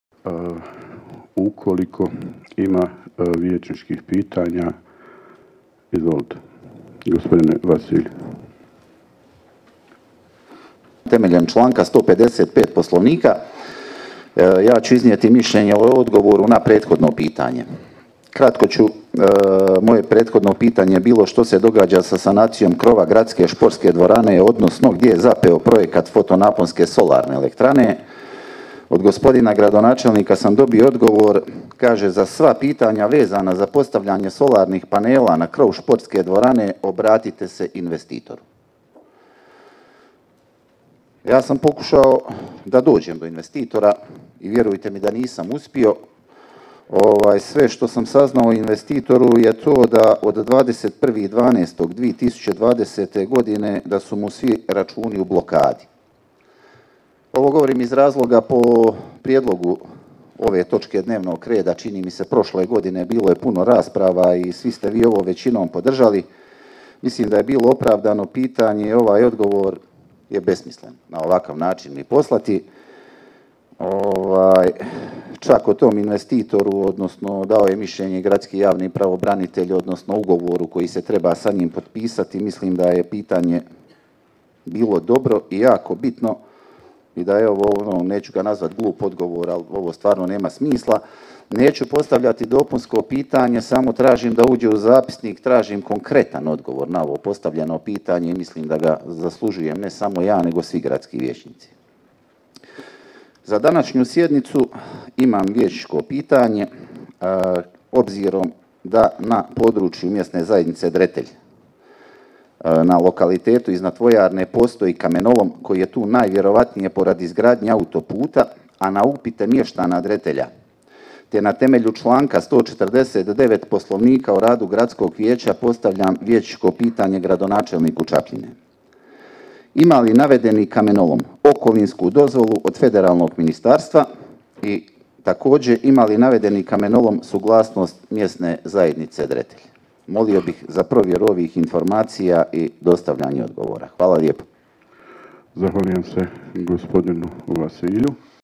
ČAPLJINA - GDJE SU ZAPELI SOLARI NA KROVU DVORANE?
Prije godinu dana, na prijedlog gradonačelnika Čapljine, usvojena je Odluka za rekonstrukciju krova športske dvorane u Čapljini, na način da je isti dodjeljen na korištinje firmi IN Solar, a za izgradnju fotonaponske solarne elektrane.
 Nakon godinu dana, solara nema ni na vidiku, dvorana i dalje prokišnjava. Štoviše, firma kojoj je dodjeljena rekonstrukcija, sanacija,  kao i postavljenje solara, skoro godinu i pol dana je u blokadi.

U blokadi je bila i onda kada joj je ovaj posao dodjeljen.

Poslušajte i pitanje, a na zahtjev mještana Dretelja, o postojećem kamenolomu u njihovom mjestu.